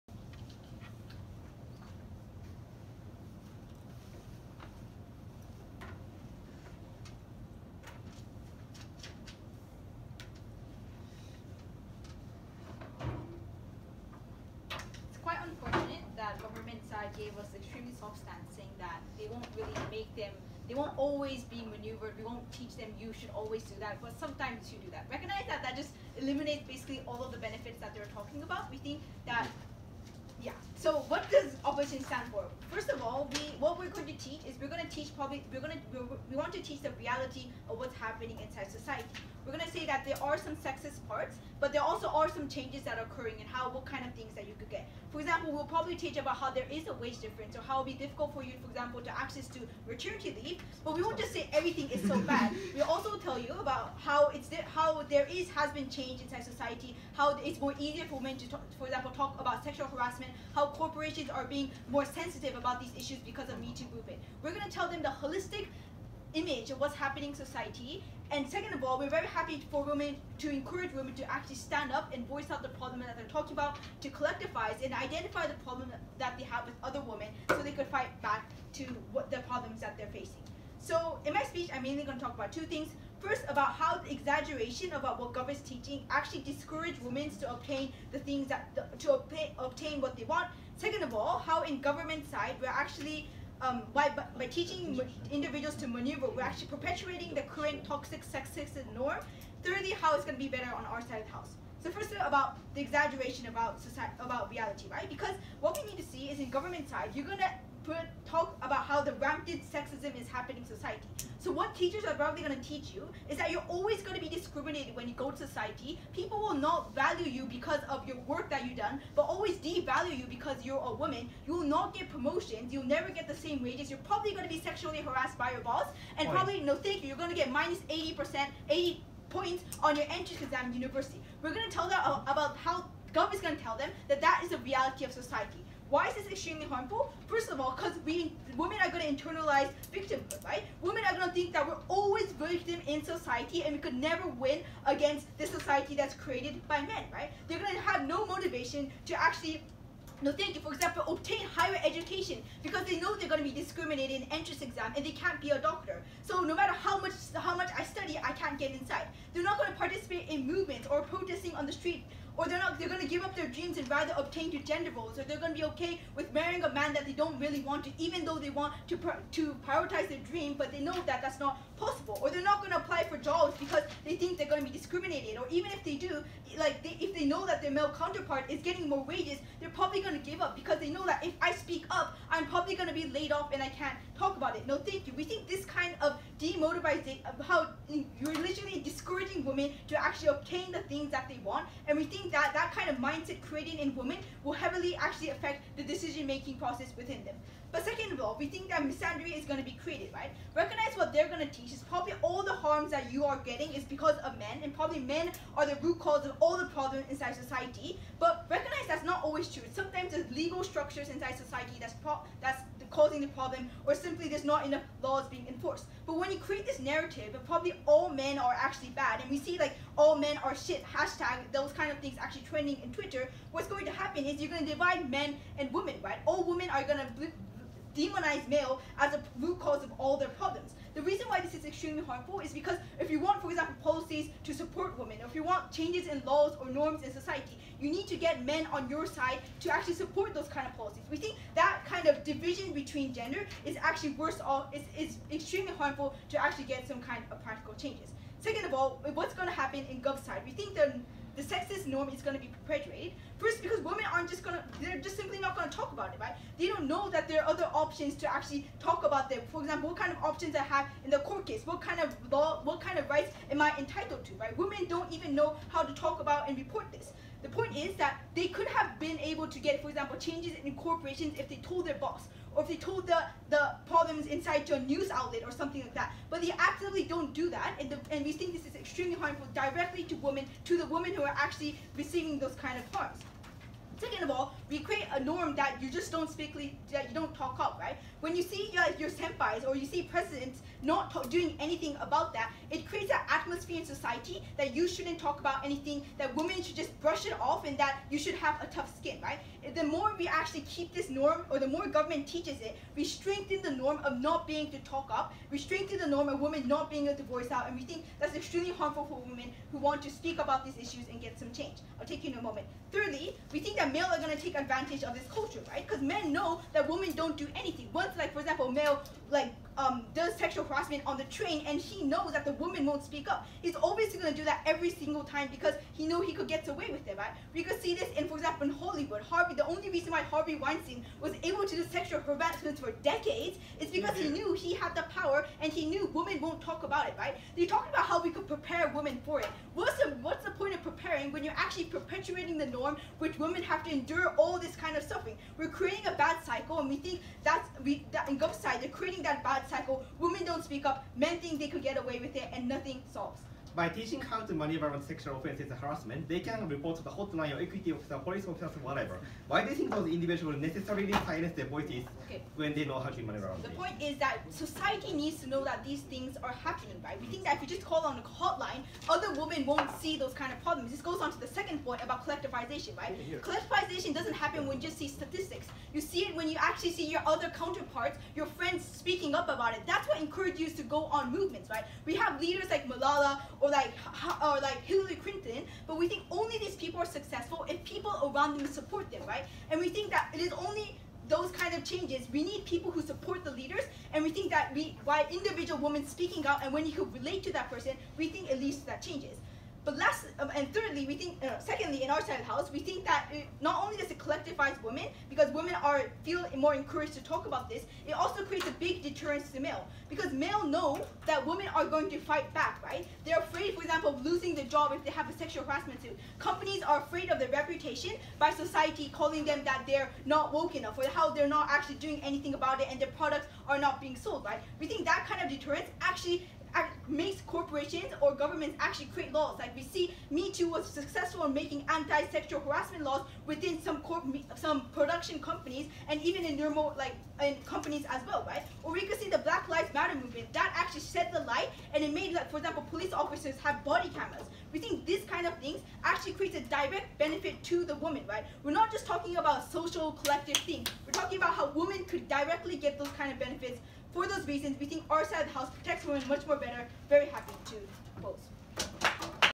The Kansai 2020 R3 LO
Leader of Opposition speech by Aika Miyazawa.